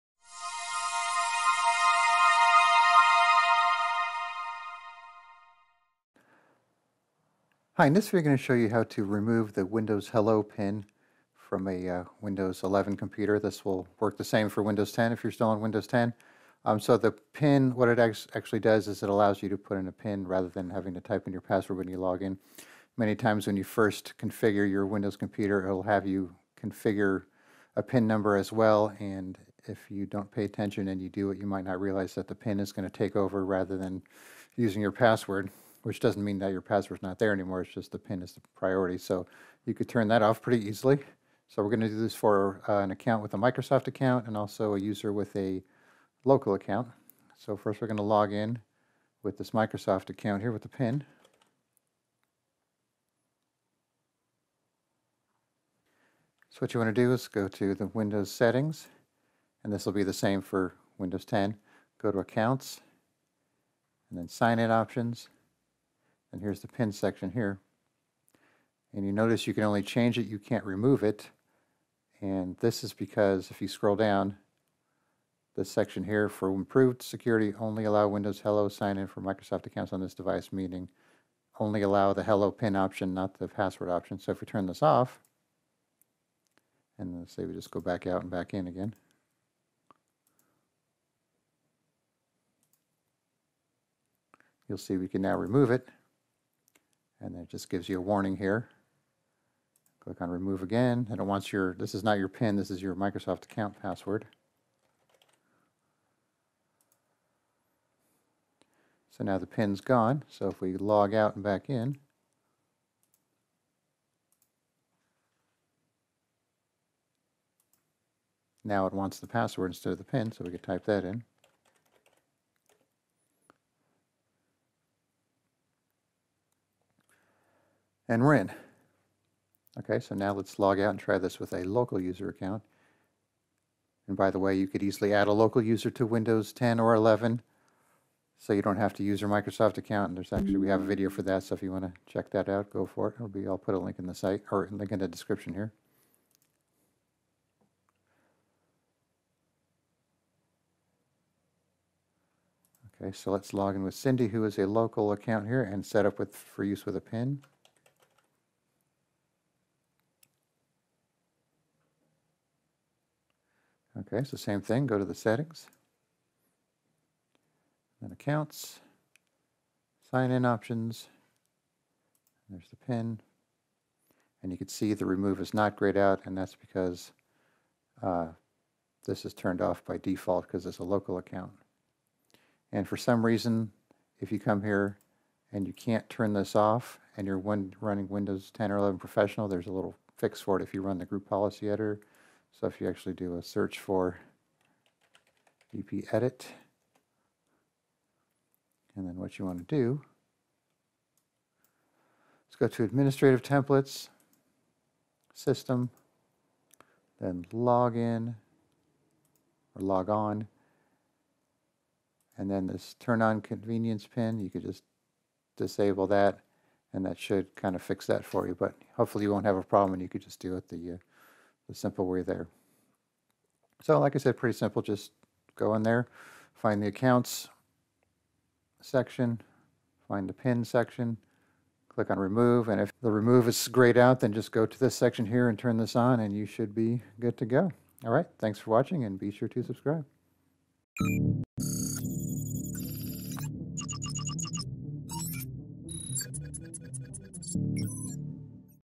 How to Remove the Windows Hello Pin So You Can Login with Your Password
TIP - You can download EaseUS Partition Master to remove Windows Hello Pin easily

When you log into a new computer for the first time or finish a new Windows installation, you will most likely be asked to create a PIN number that you can use to login with rather than your password. You might not even realize that this PIN will be used in place of your password and that might not be what you want.

Then when you go to remove your PIN, you might find that the Remove button is greyed out, not allowing you to remove it. Fortunately, this is an easy fix and you just need to change the Hello sign in options in the Windows settings. If you are using Windows Professional Edition then you can even use the Group Policy Editor to help fix this problem. This demo shows how to do this with a Microsoft account as well as a local user account and was made with Windows 11 but the procedure works exactly the same for Windows 10.

Here is how you create a local account in Windows 11 (and Windows 10).